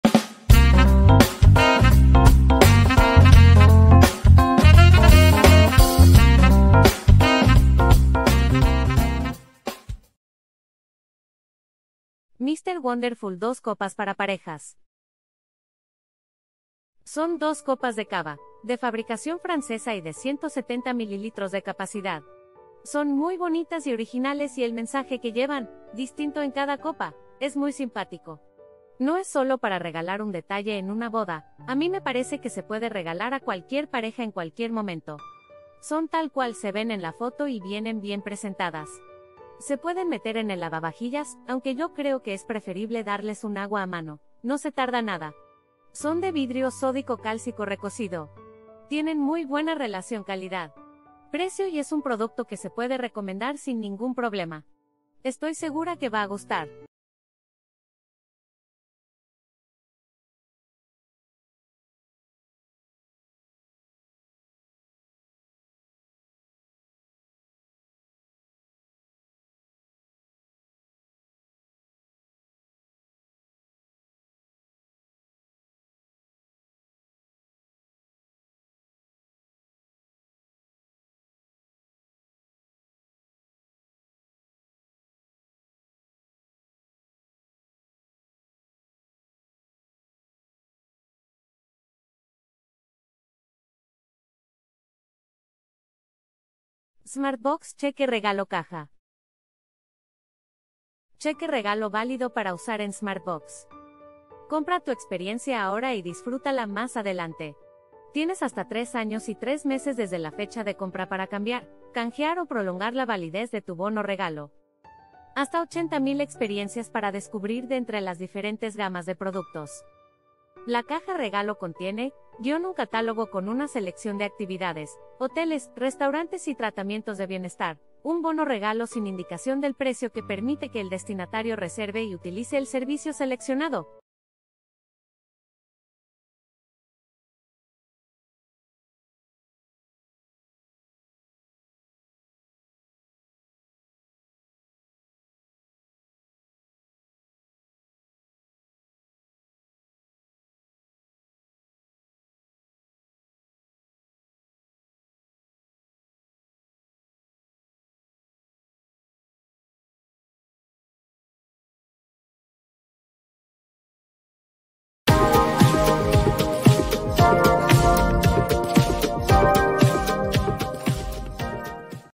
Los MEJORES REGALOS para DOS del 2021 🎁
Hoy os presentamos "Smartbox Cheque Regalo Caja" y "Mr. Wonderful Dos copas para parejas".

👉 PRODUCTOS DEL VIDEO:

[00:11] ► Smartbox Cheque Regalo Caja

[00:51] ► Mr. Wonderful Dos copas para parejas